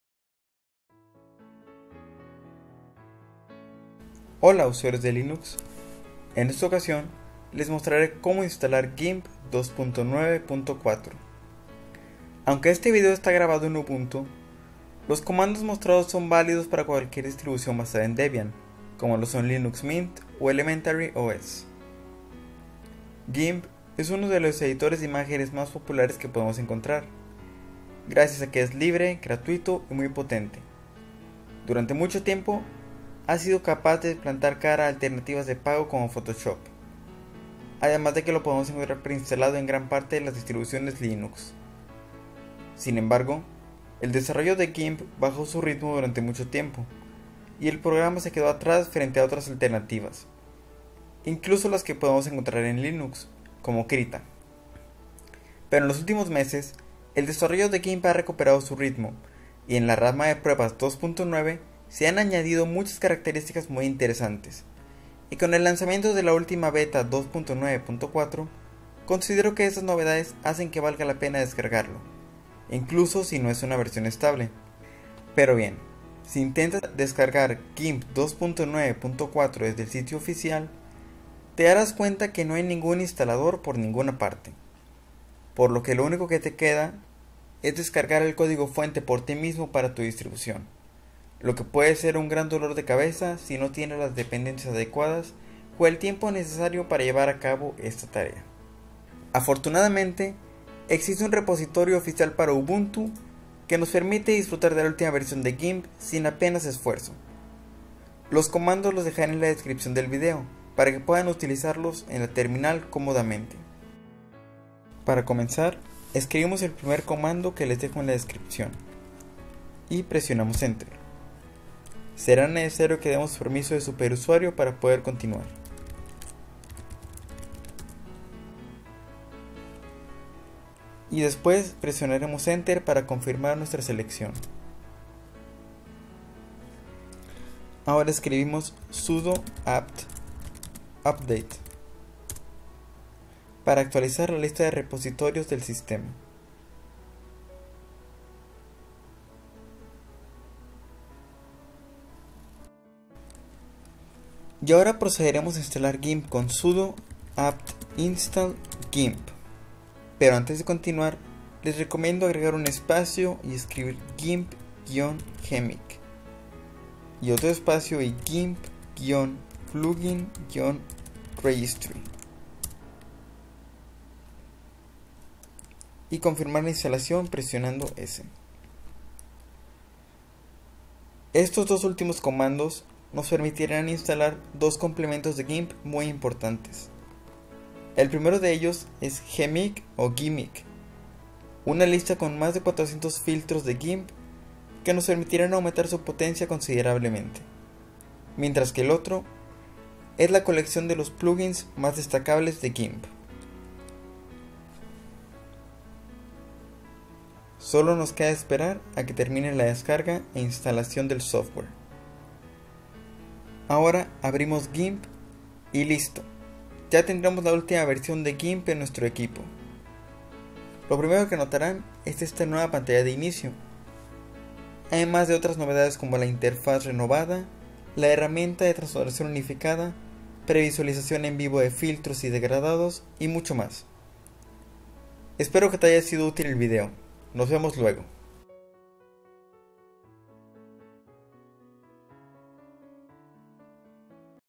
Como instalar Gimp 2.9.5 + G'MIC en Ubuntu | Technodyan
Gimp es el editor de vídeos libre más popular, y en su última versión incorpora muchas novedades que lo ponen de nuevo en la órbita de los mejores editores de fotos. Aquí te enseño como instalarlo en Ubuntu y demás derivados de Debian.

Los comandos mostrados en el vídeo:
- sudo add-apt-repository ppa:otto-kesselgulasch/gimp-edge
- sudo apt update
- sudo apt install gimp

Recuerda visitar mi blog donde encontrarás tutoriales, análisis, reseñas, opiniones y artículos sobre Linux, Android y Windows:
- Technodyan.com

¡Gracias!